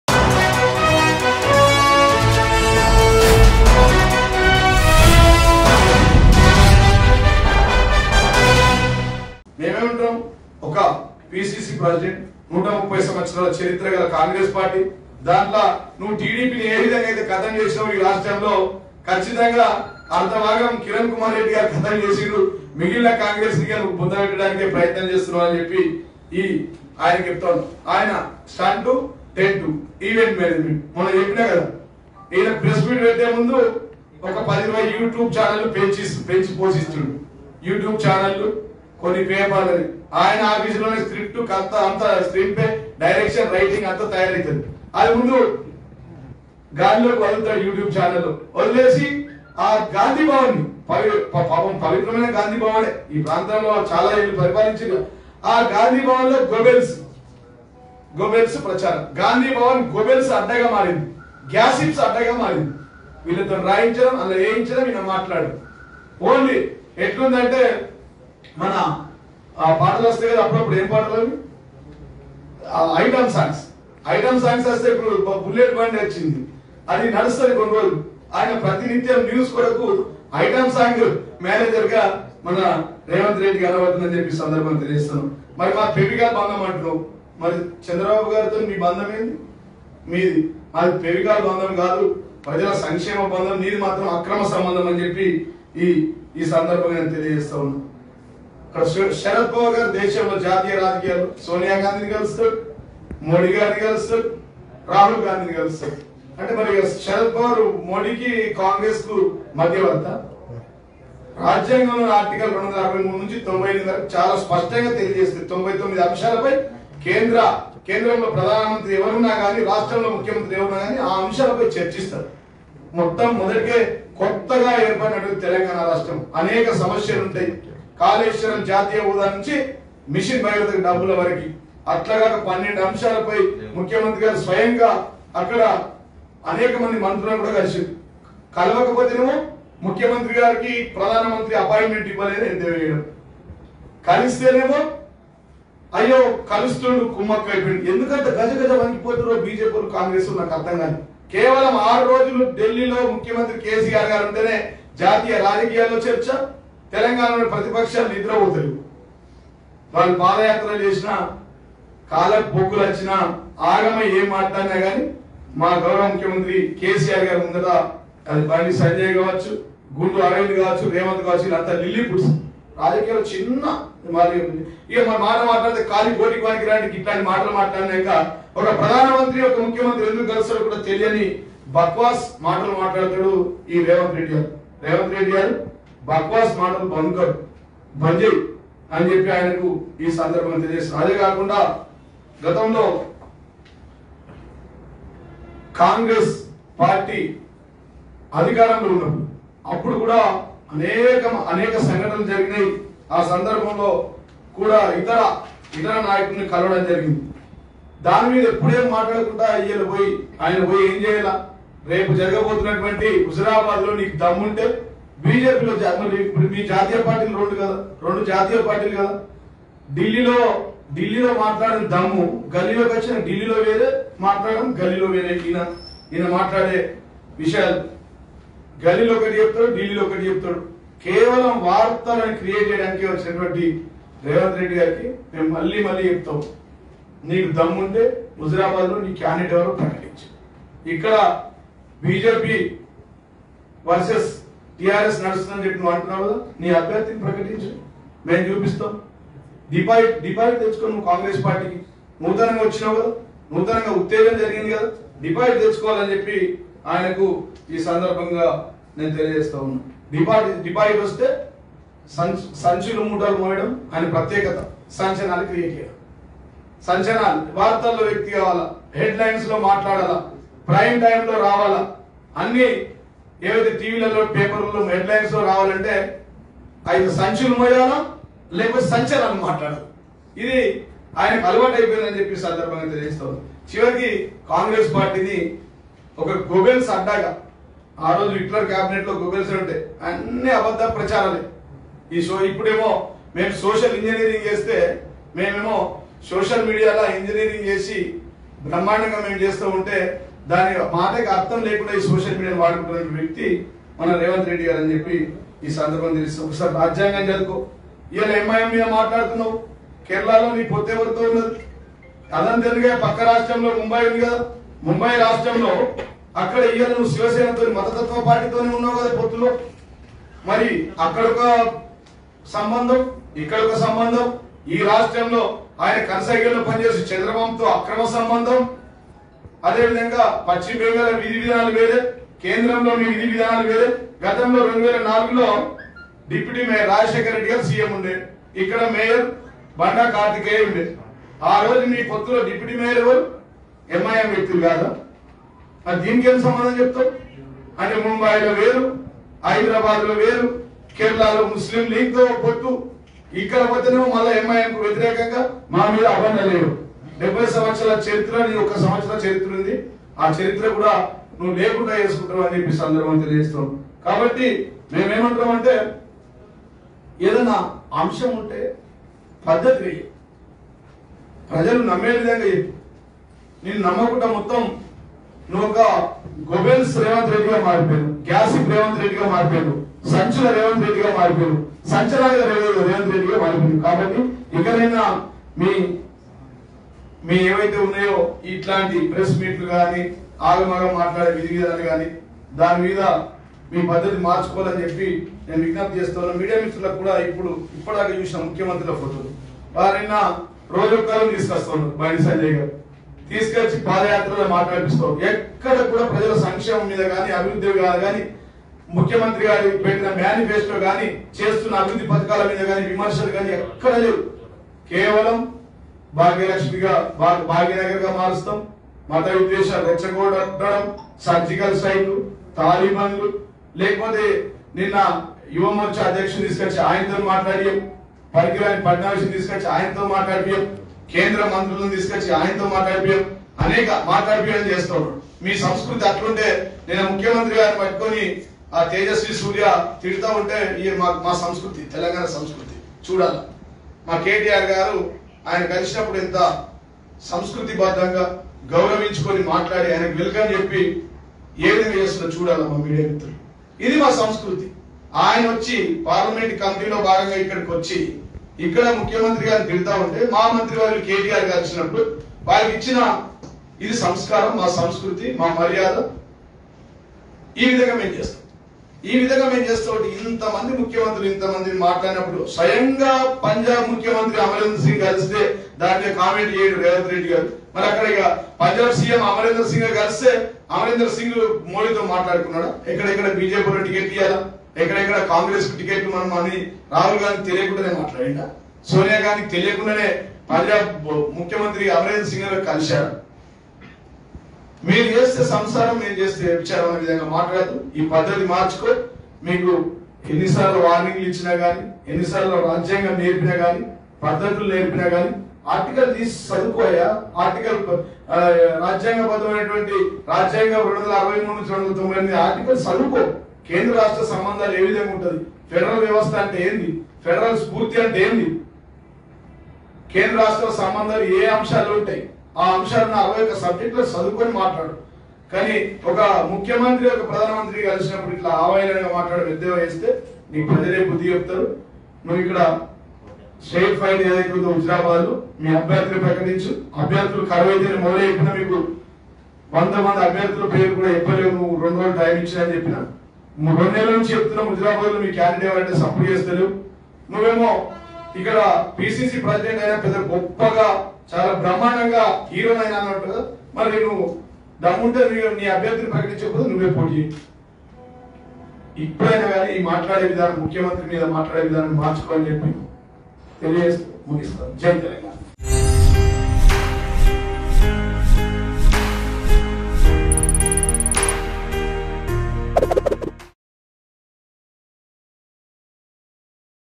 TRS MLA Jeevan Reddy Open Challenge To Revanth Reddy Over Huzurabad By-Election | Mango News
TRS MLA Jeevan Reddy Open Challenge To Revanth Reddy Over Huzurabad By-Election.